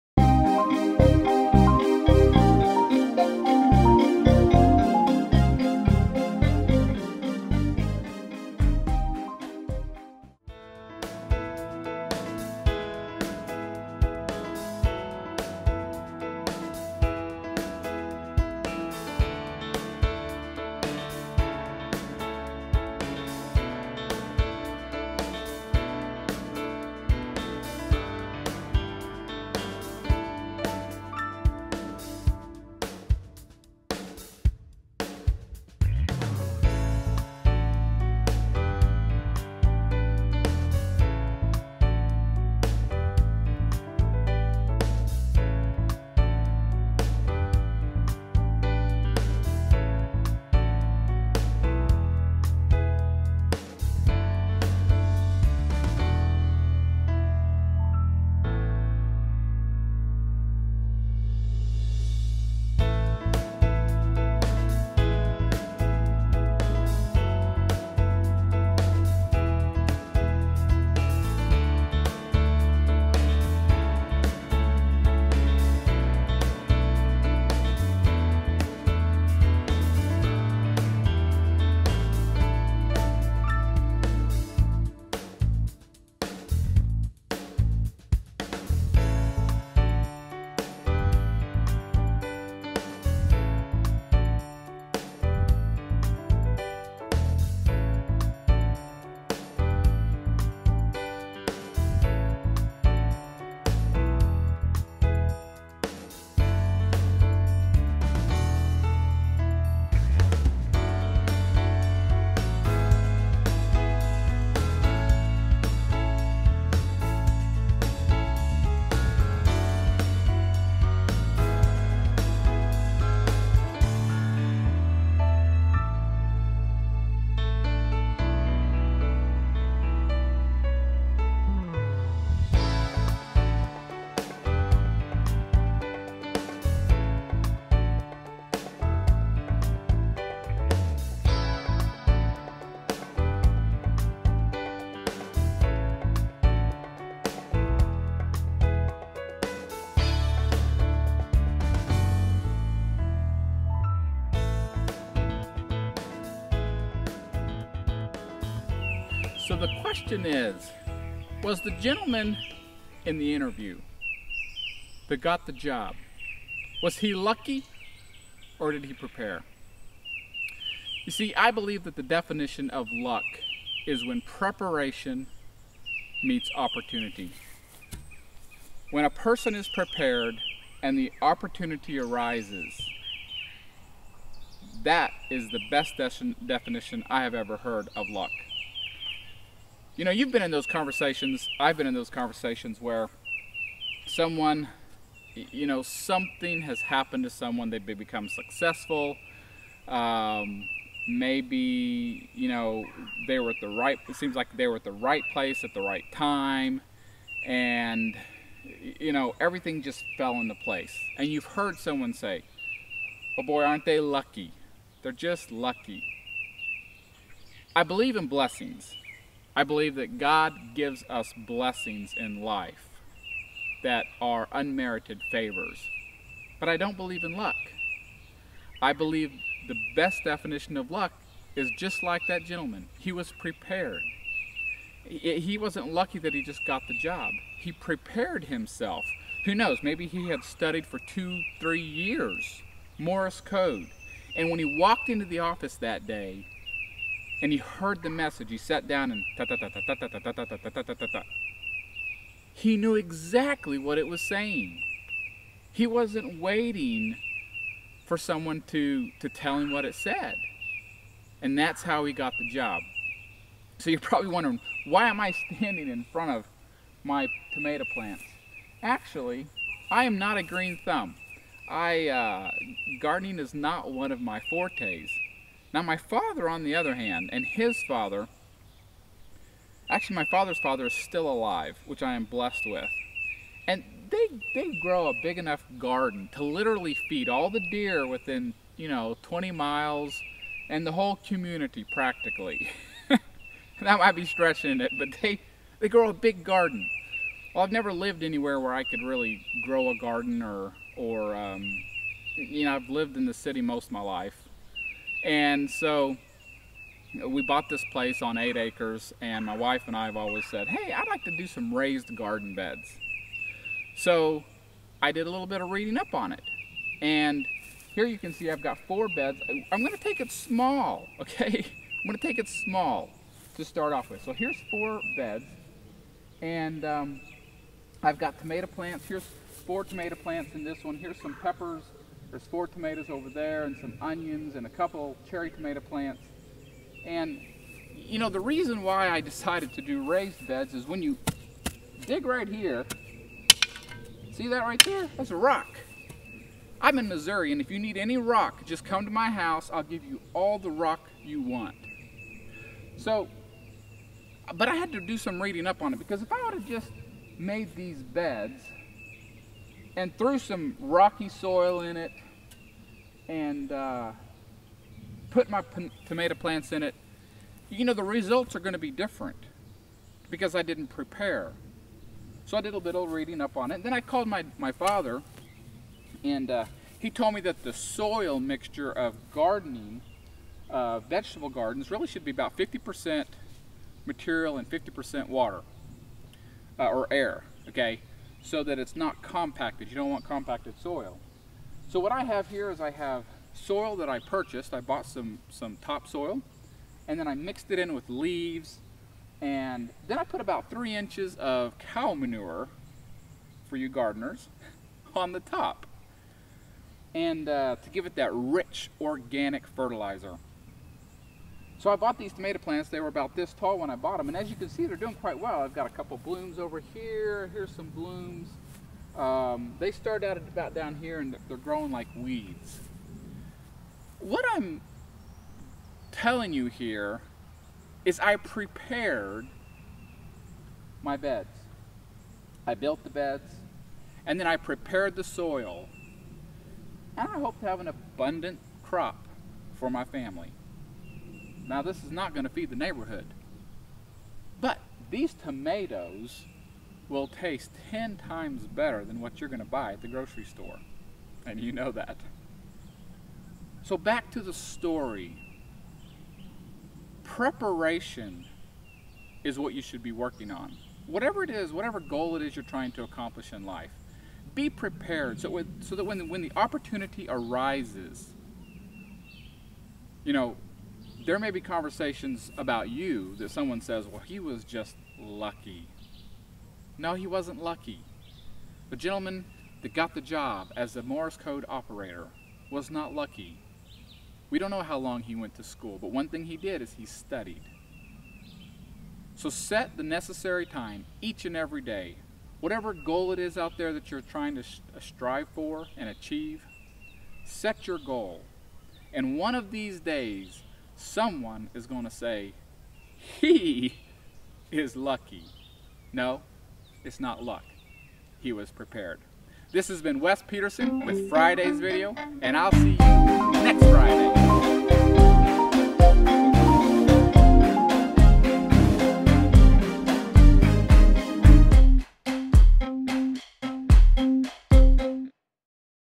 The Telegraph Interview
In this video you will see an interview that paid off.  Some people are called LUCKY ... yet this video explains that LUCK is when preparation and opportunity converge.